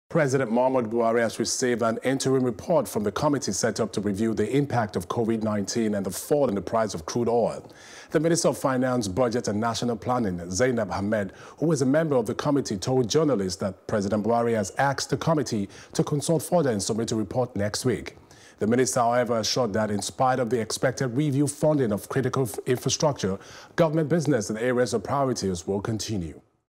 Convid19: Buhari Recieves Interim Report From Committee
President Muhammadu Buhari has received an interim report from the committee set up to review the impact of Covid 19 and the fall in the price of crude oil.